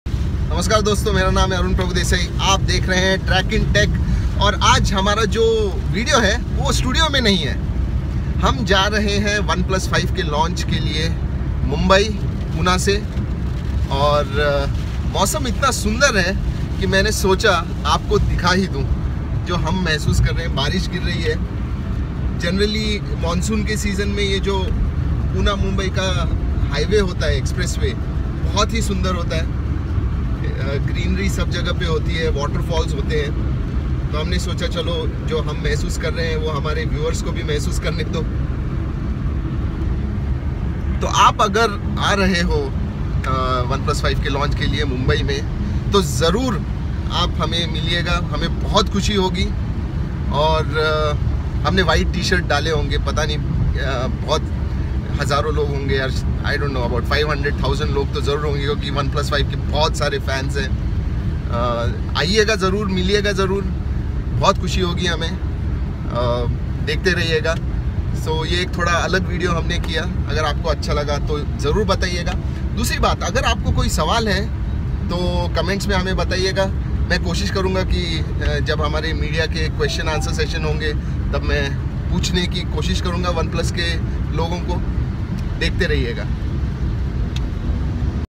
Oneplus 5 - On our way to Oneplus 5 event at mumbai
Thoda alag video ...hum oneplus 5 event ko ja rahe hai aur mausam badaa sundar hai...toh socha aapke saath bhi share karen Trakin Tech 100k Subscriber Mega Giveaway Credits
Source: